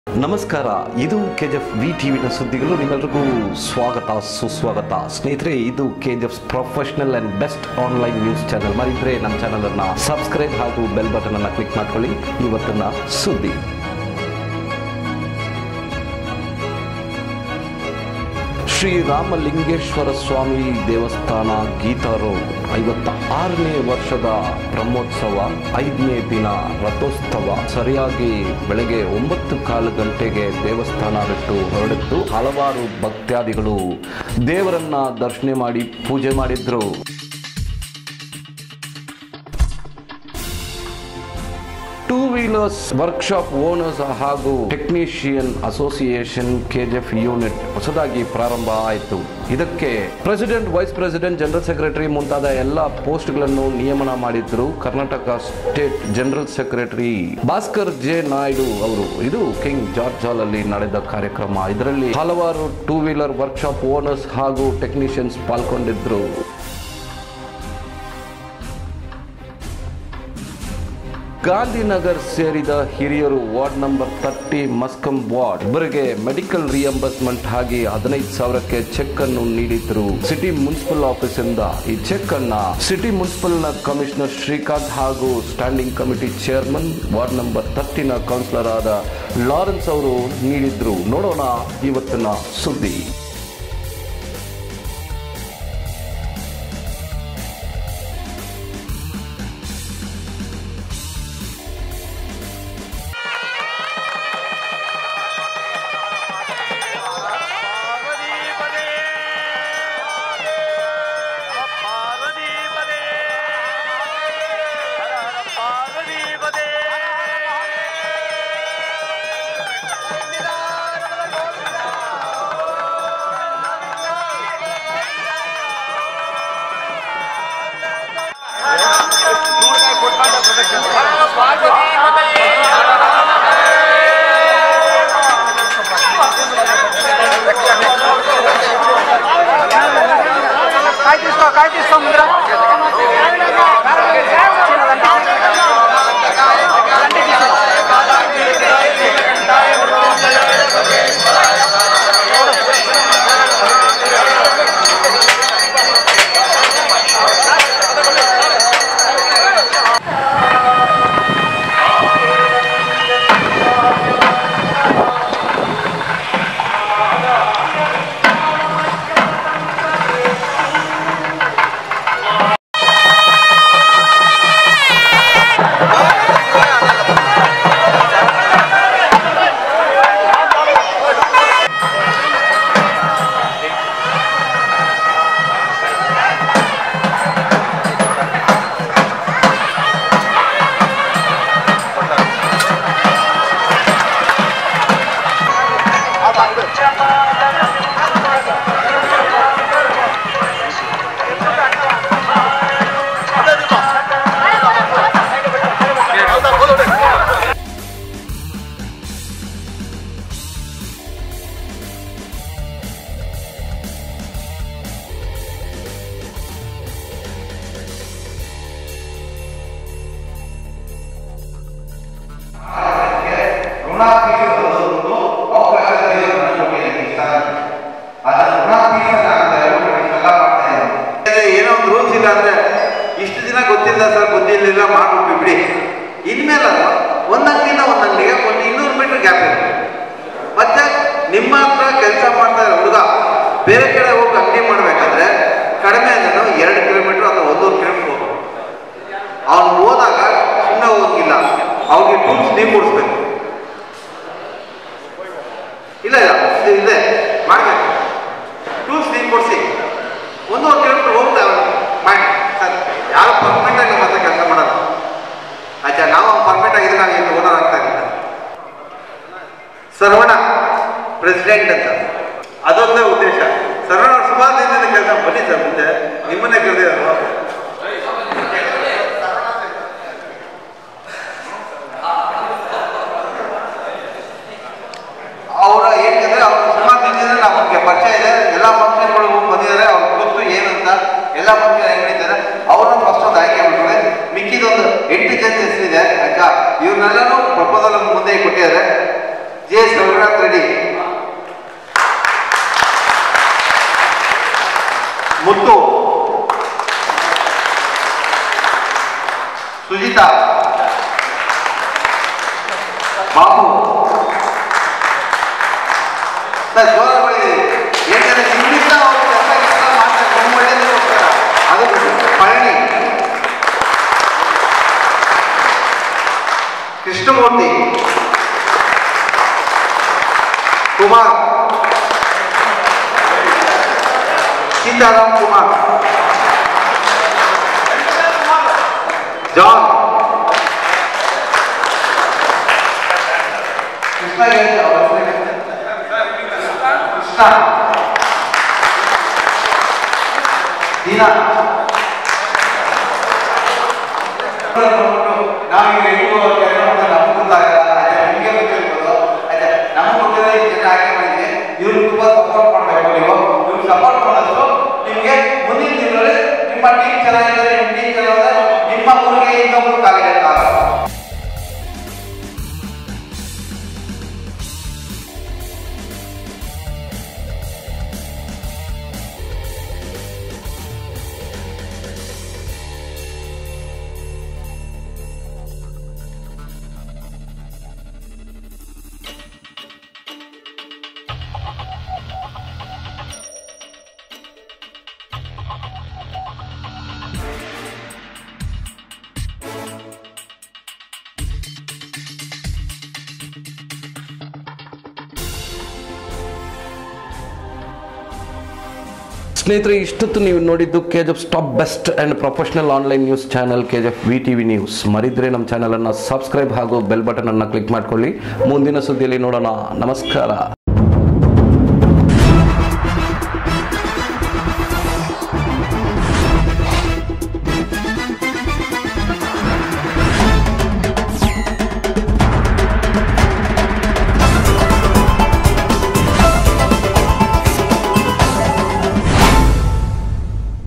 KGF VTV NEWS|15000/- Cheque issued by CMC| New Association Formation
KGF VTV NEWS DETAILS:
1 Sri Ramlingeshwarswamy Temple 56th year Brahmotsavam today is the 5th day and grand Rathotsavam chariot took palace from Geetha Road.
2. Two wheeler workshop Owners & Technicians Association formation took place at King George hall.
3. CMC Commissioner Srikanth & Ex Standing Committee Chairman & Ward No 30 Muskam Municipal Councilor Lawrence issued Rs 15000/- Cheque to Senior citizen of Gandhi Nagar resident
KGF VTV NEWS TEAM